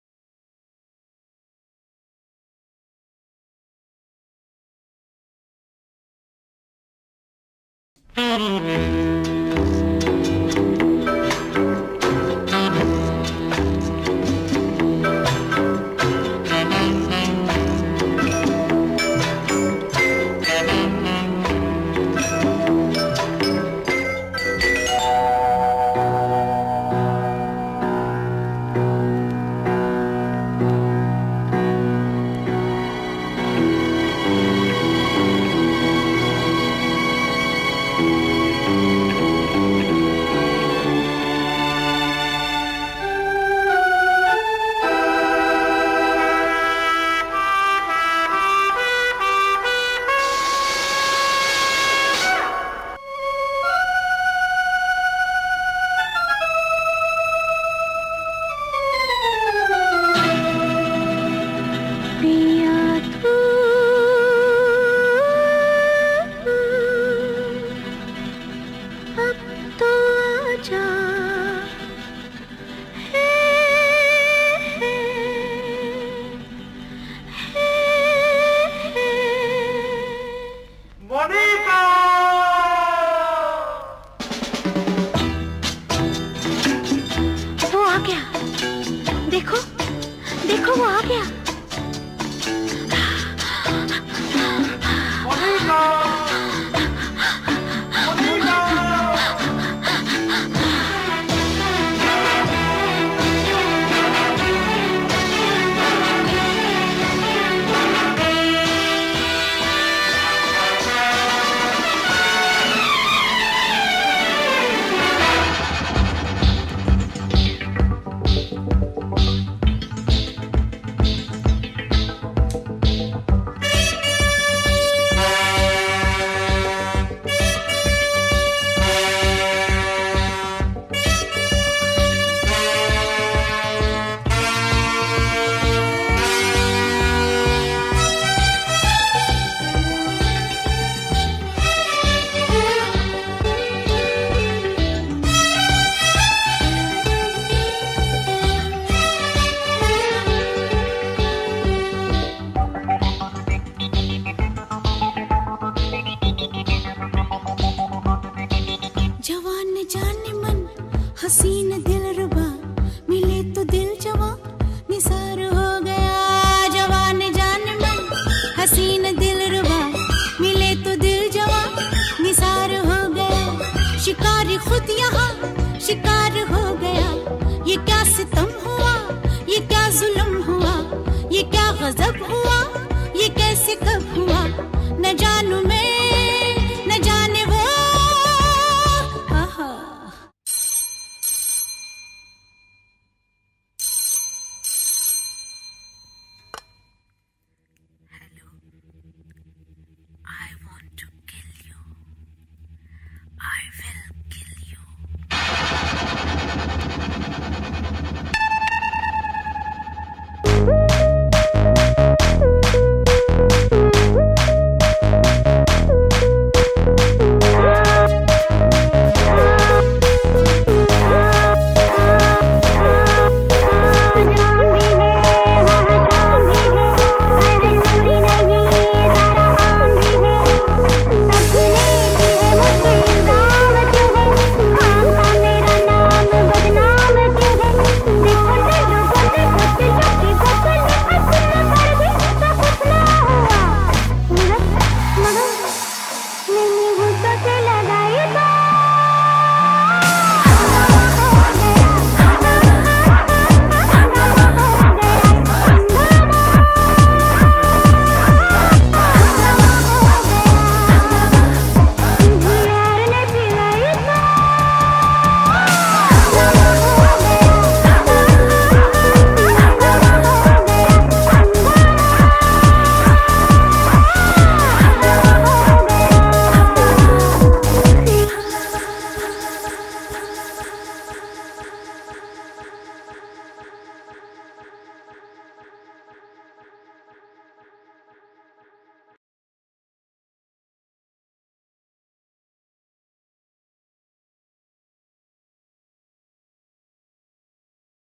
BANJARA SCHOOL OF DANCE - BELLYWOOD DIVA
is any bellydance festival complete without some crazy bollywood tunes!
choreographed by damini sahay for the beginners batch at the 6th edition of jashn-e-banjara

For Class or Event Queries : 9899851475